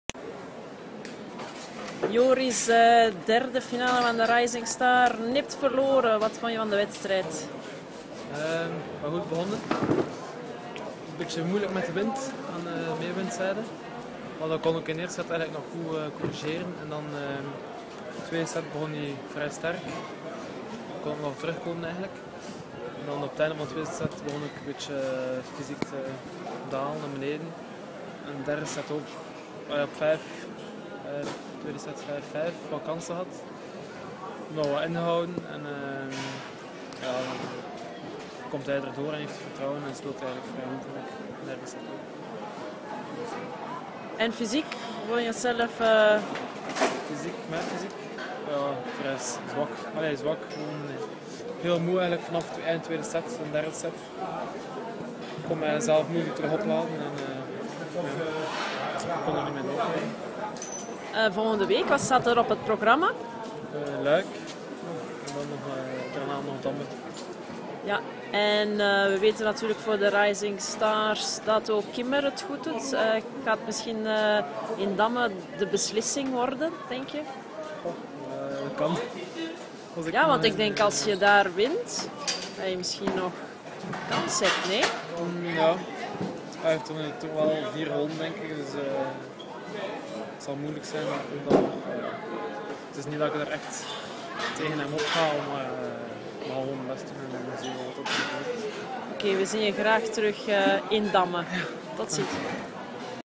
Joris De Loore lors de la finale au Rising Stars de Coxyde
Bravo à Joris De Loore qui a réussi à atteindre une 3e finale aux Rising Stars ! Notre compatriote a malheureusement été battu par le Français Gregoire Barerre (3-6, 7-5, 6-3) lors de la finale à Coxyde. Bravo à ces jeunes talents qui nous offrent d'excellents moments de tennis !